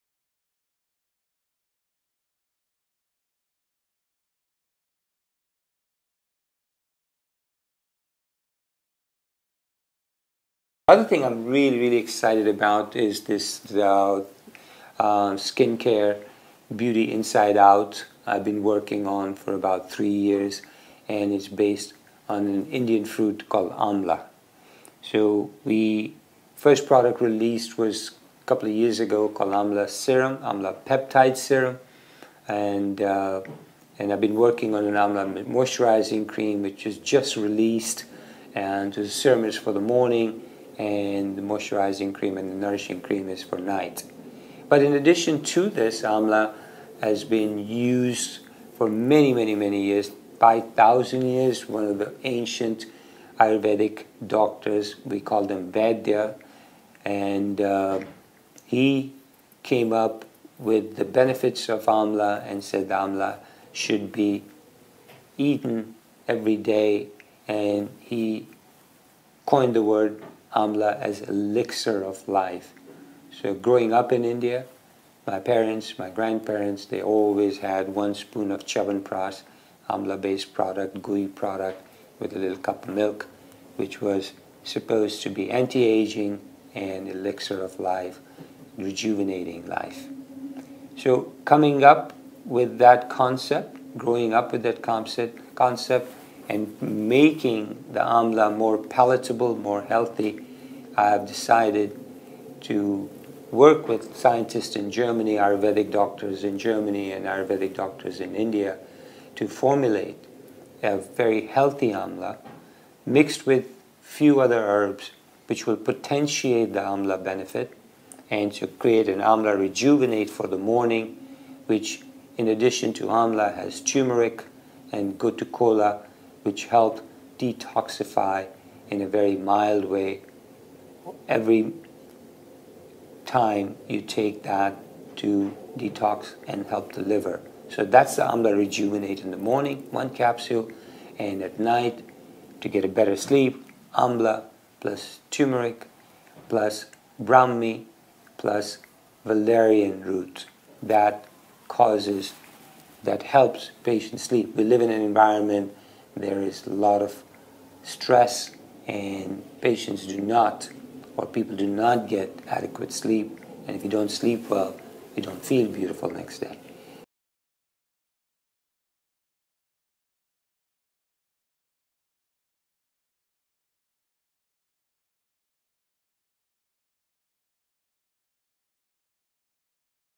Dr. Kanodia discusses his new skincare line- AMLA
Dr. Kanodia Discusses hes new skincare line- AMLA